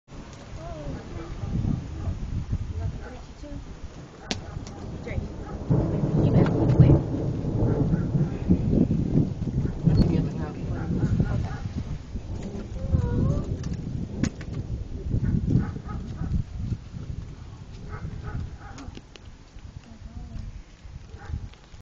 Tornadoes Wall Cloud May 25, 2010, Pulaski County, AR
It never did touch down but it was a scary sight, it was just over Southwest Little Rock at this time.